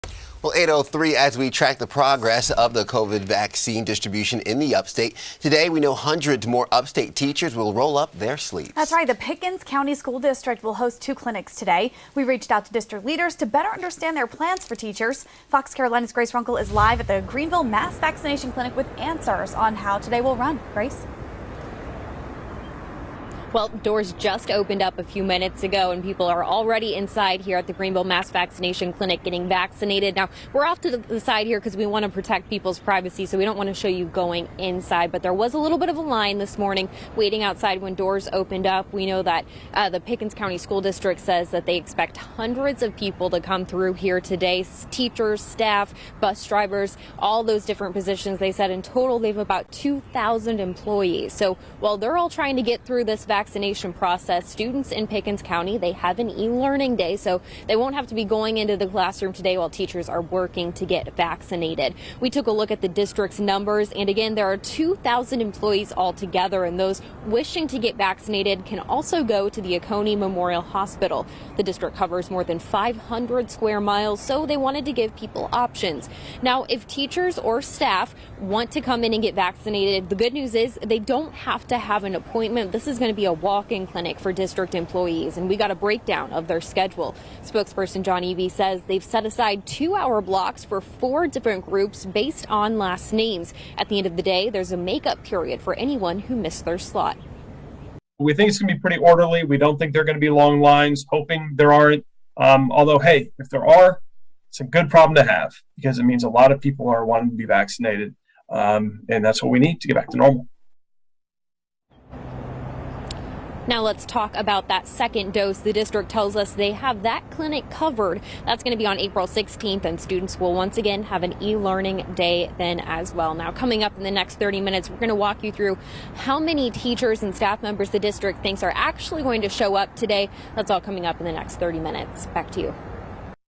Pickens Co. teachers getting shots today
Officials in Pickens County say about 14,000 doses of the Pfizer vaccine have been set aside for employees in the Pickens Co. School District.